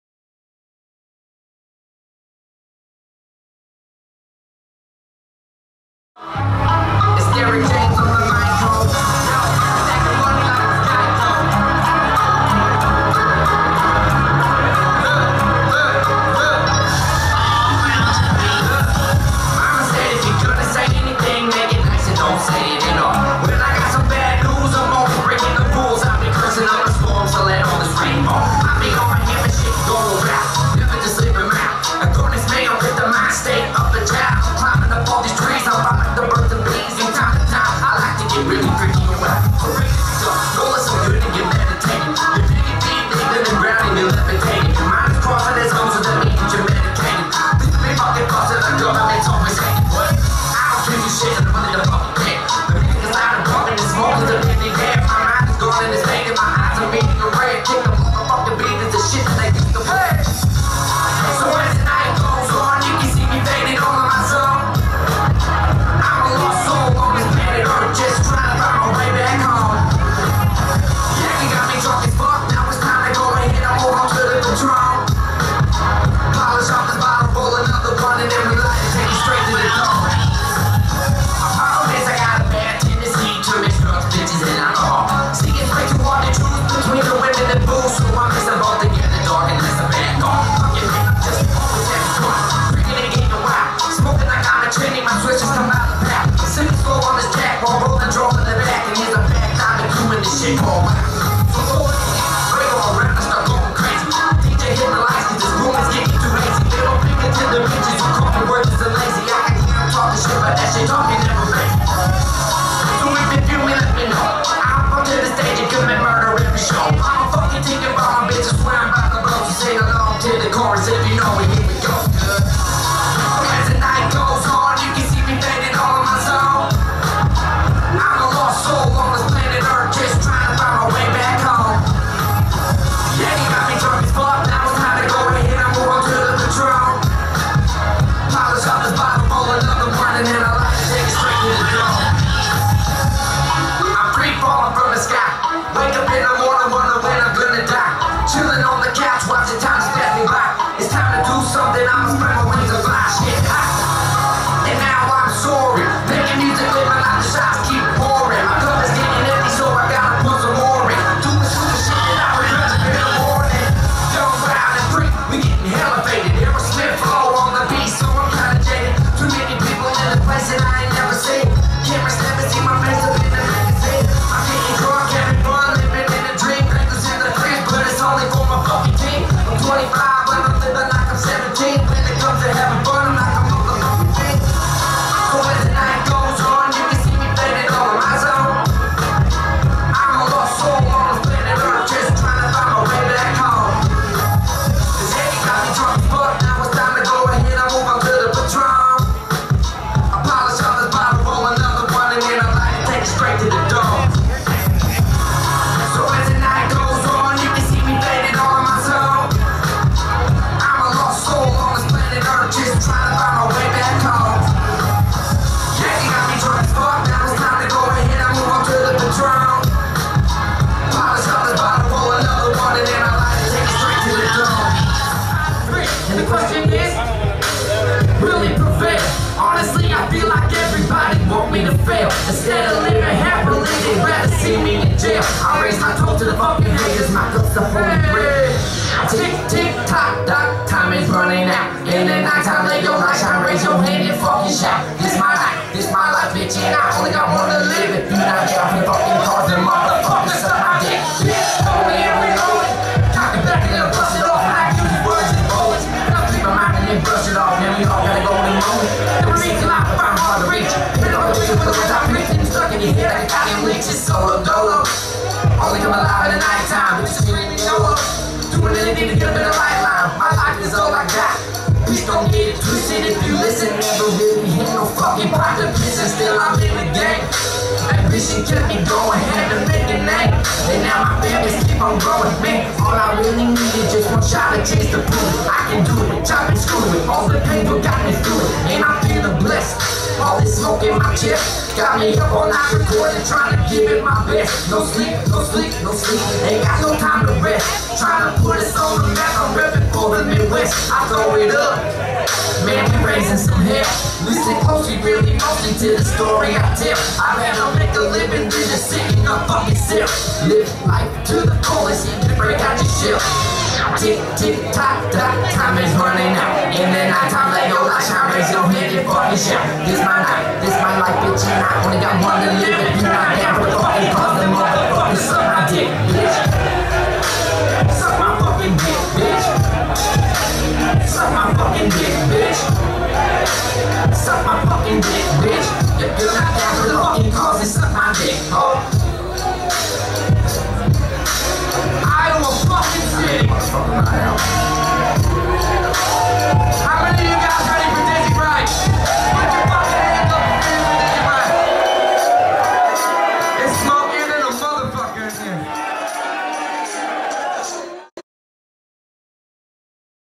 DEREK JAMES - LIVE PERFORMANCE @ BLUE MOOSE TAP HOUSE IN IOWA CITY
Derek James opens up for Dizzy Wright at Blue Moose Tap House In Iowa City ( 3-2-16 )